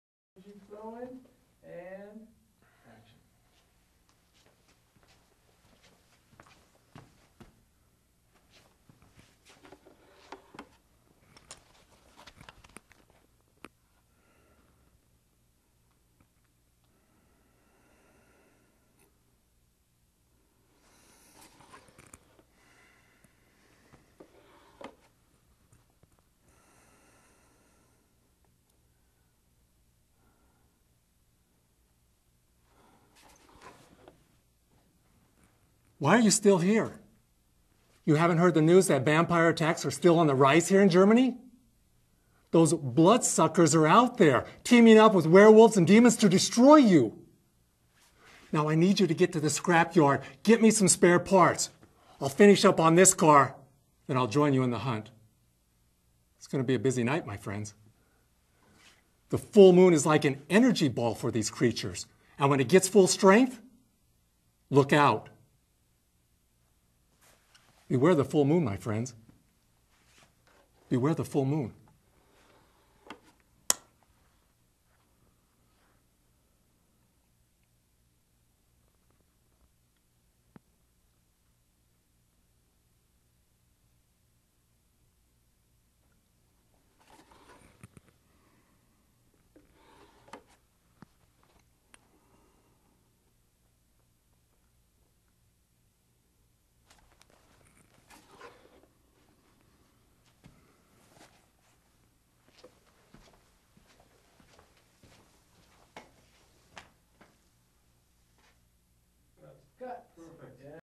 John Cruz Actor Utah Van Helsing Vampires in Germany
Pre- Graphics green screen shoot for Van Helsing Theme Park